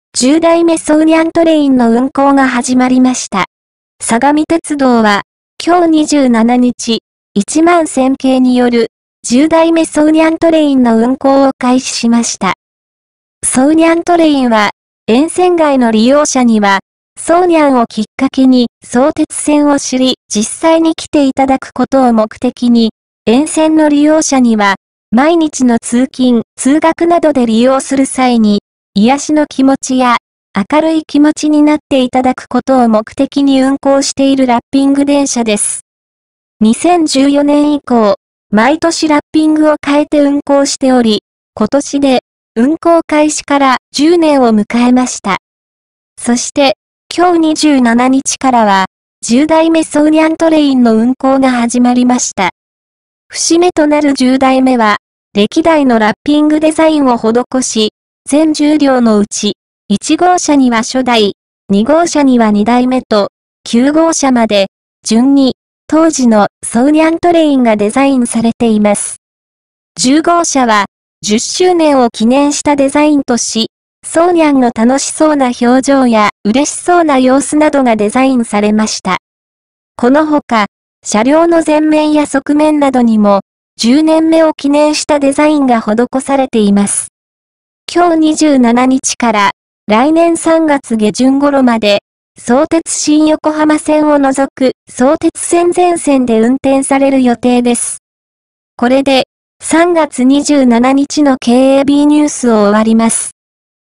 【今年で10年目】相鉄11000系「十代目そうにゃんトレイン」が運行開始(2023年3月27日ニュース)
訂正【テロップ】1:27～1:38
正しくは、2024年3月下旬までの運行予定です。

　「十代目そうにゃんトレイン」の運行が始まりました。
　相模鉄道は、今日27日、11000系による「十代目そうにゃんトレイン」の運行を開始しました。
　そうにゃんトレインは、沿線外の利用者には、そうにゃんをきっかけに相鉄線を知り実際に来ていただくことを目的に、沿線の利用者には、毎日の通勤・通学などで利用する際に、癒しの気持ちや明るい気持ちになっていただくことを目的に運行しているラッピング電車です。
　2014年以降、毎年ラッピングを変えて運行しており、今年で運行開始から10年を迎えました。
　そして、今日27日からは、十代目そうにゃんトレインの運行が始まりました。
　節目となる十代目は、歴代のラッピングデザインを施し、全10両の内、1号車には初代、2号車には二代目と、9号車まで順に当時の「そうにゃんトレイン」がデザインされています。10号車は、10周年を記念したデザインとし、そうにゃんの楽しそうな表情やうれしそうな様子などがデザインされました。
　この他、車両の前面や側面などにも10年目を記念したデザインが施されています。
　今日27日から来年3月下旬ごろまで、相鉄新横浜線を除く相鉄線全線で運転される予定です。